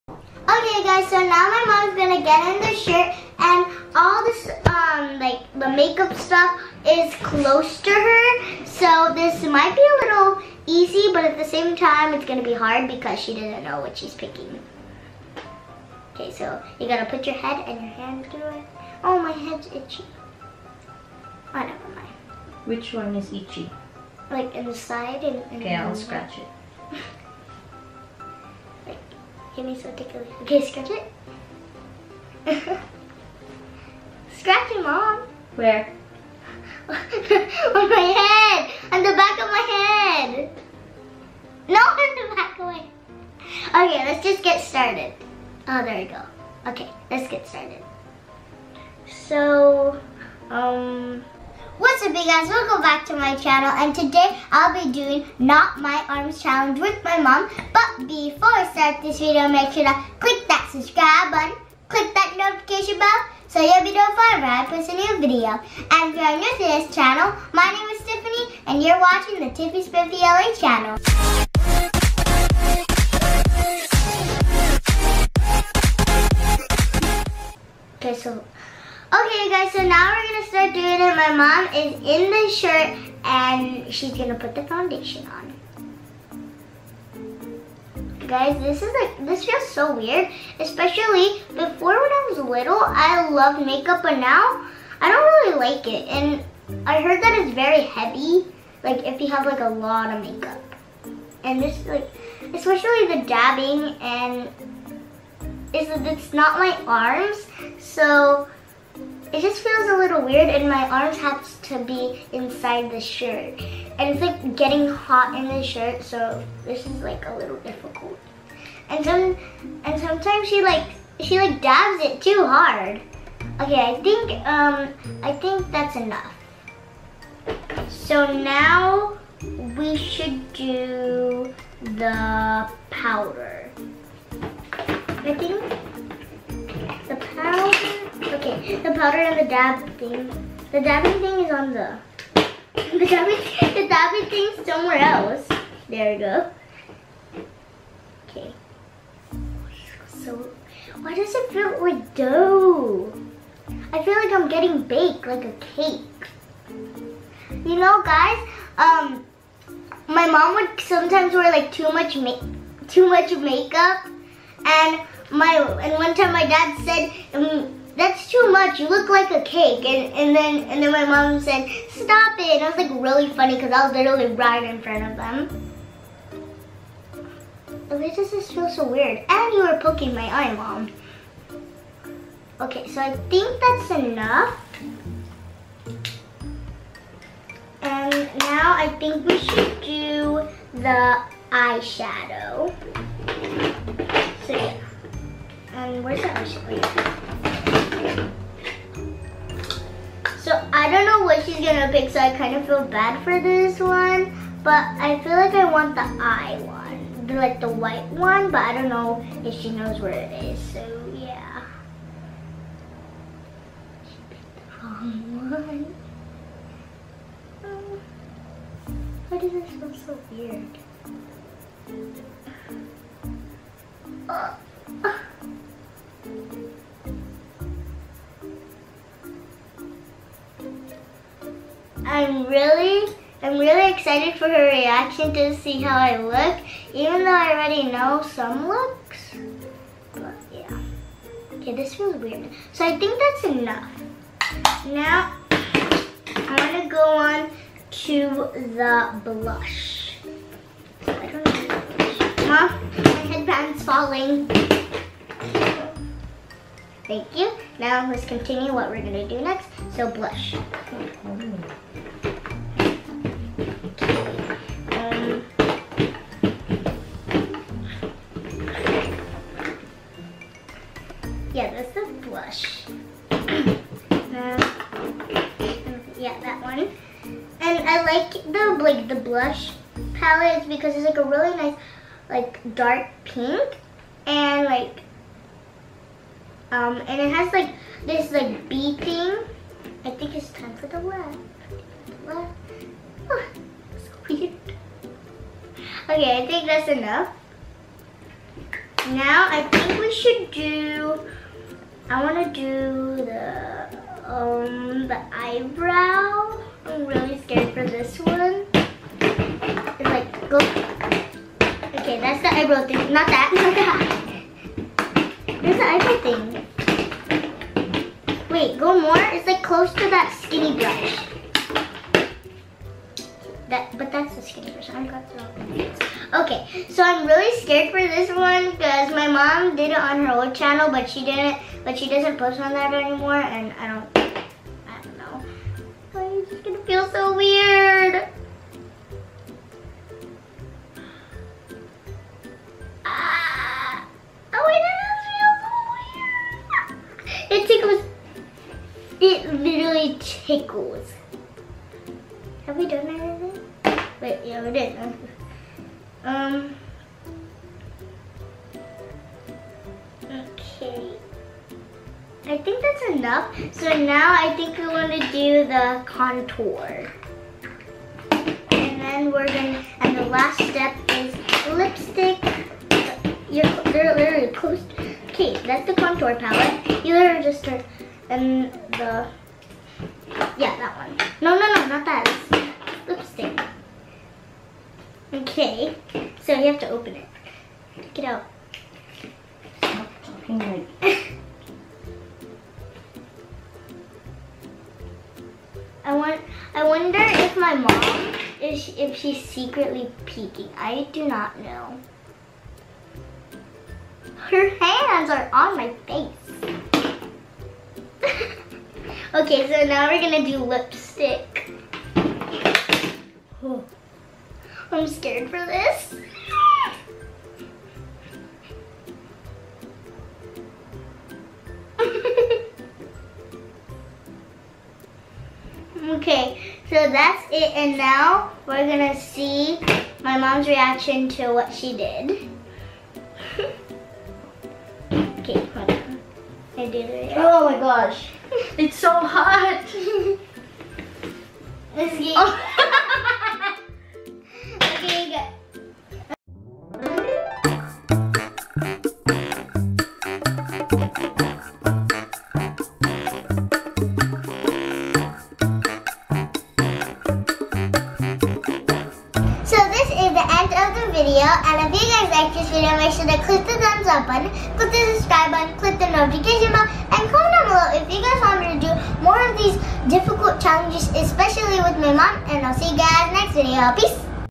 NOT MY ARMS CHALLENGE (gone wrong) | Tiffy Spiffy LA
Hey Spiffy Fam, In this video I did a fun challenge with my Mom called Not My Arms Challenge. Enjoy! :)